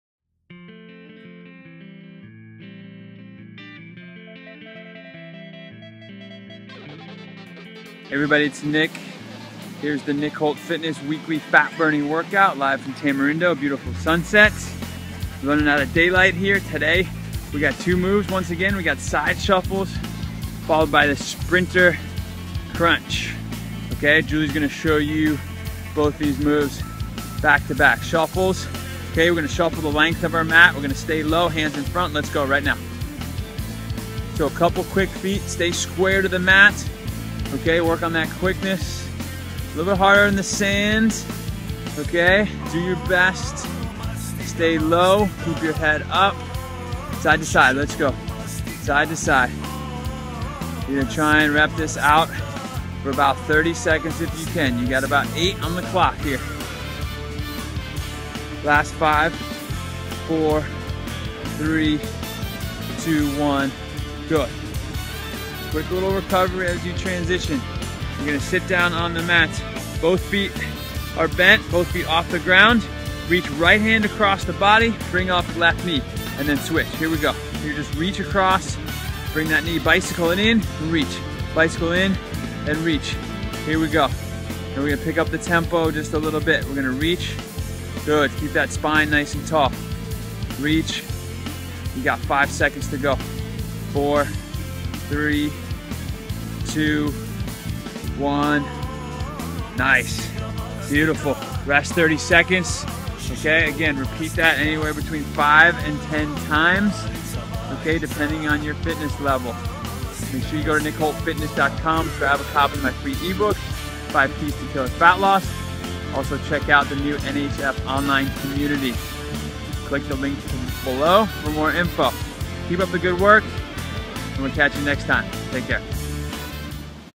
Nick Holt Fitness Fat Burning Workout: Side Shuffles & Sprinter Crunches
NHF Weekly Fat Burning Workout: 30 seconds of Side Shuffles and 30 seconds of Sprinter Crunches. For maximum results, make sure to repeat this set 5x - 10x. For more exercises, nutrition tips, and your free copy of our e-book: 5 Keys To Killer Fat Loss, head over